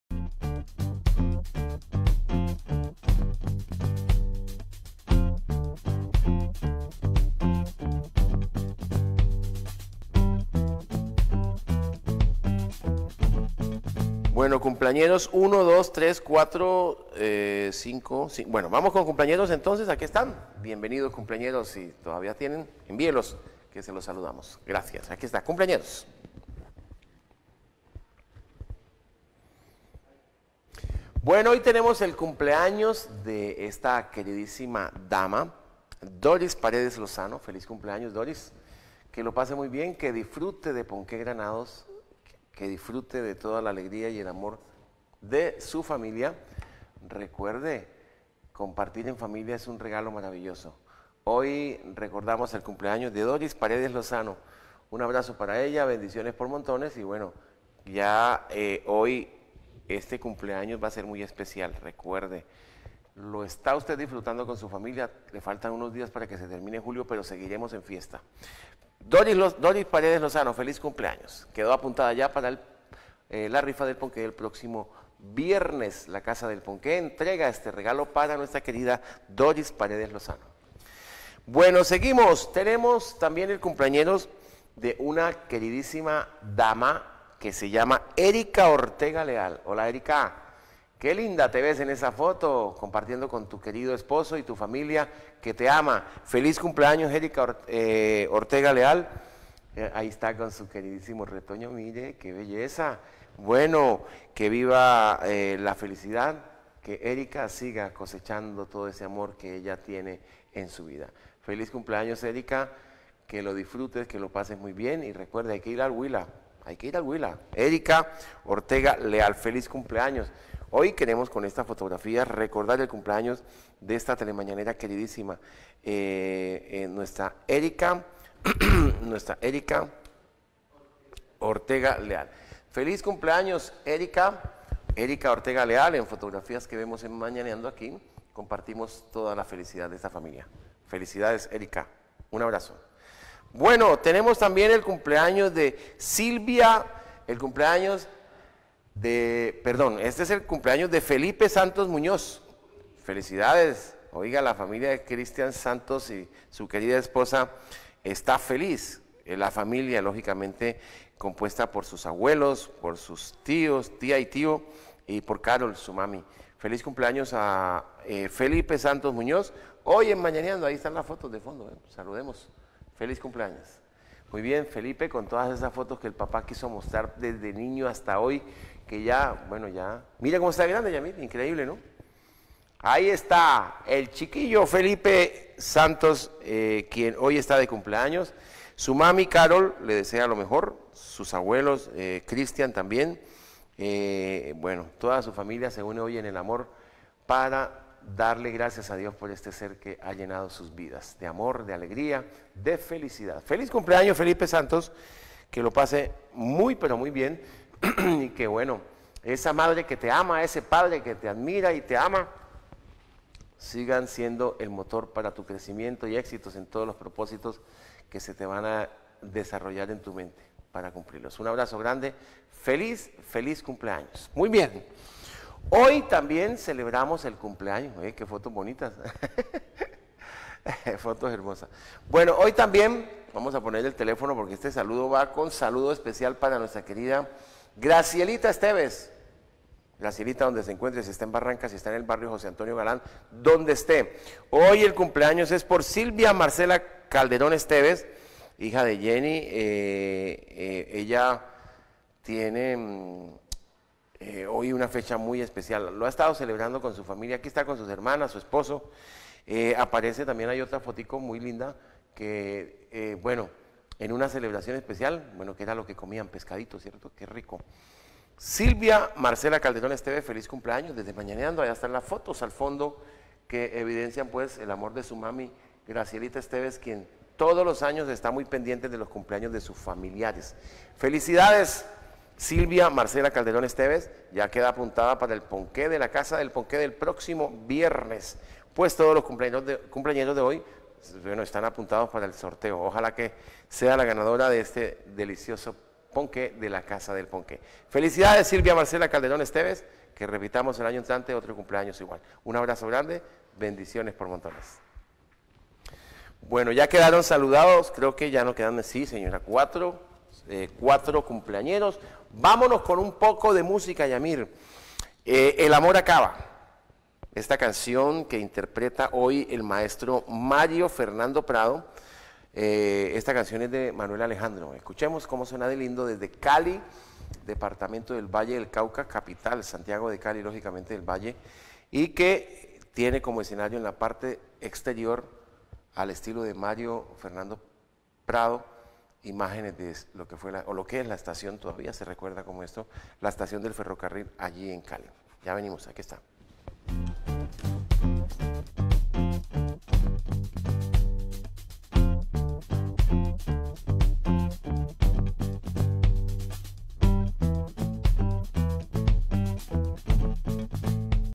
Feliz cumpleaños
Felicidades a: Doris Padres Losano, Erika Ortega Leal, Felipe Santos Muños, Silvia Marcela Calderón Estevez.